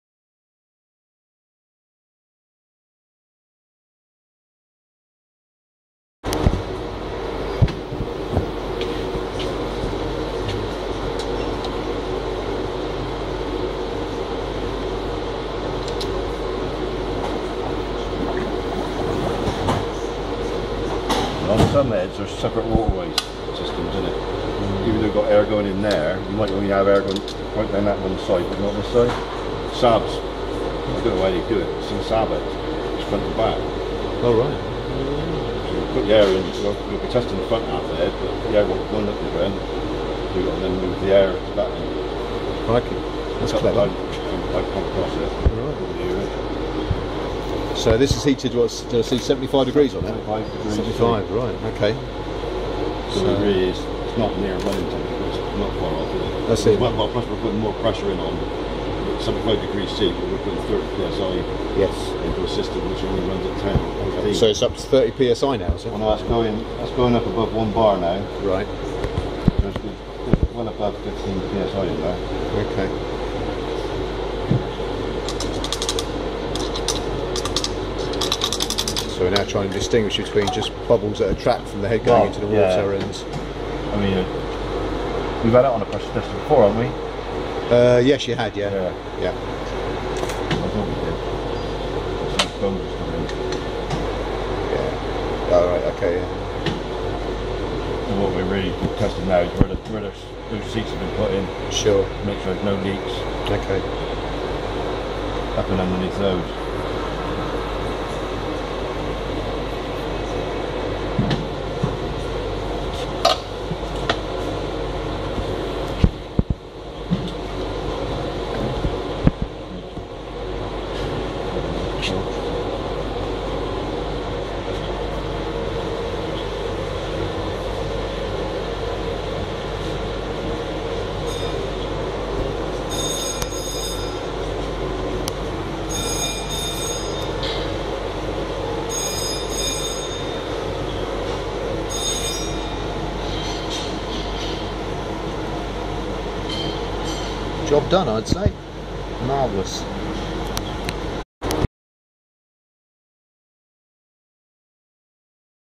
Cylinder head crack test
From our cylinder head Masterclass feature in issue 219. The cylinder head is a cast iron head for Triumph TR4. These heads are prone to heat stress cracks but they can be repaired as proven in our feature. Here, the head is pressurised to 20psi or more and submerged in a heated tank of water at 75C. All engineering done by Hurley Engine Services, Bath, Somerset.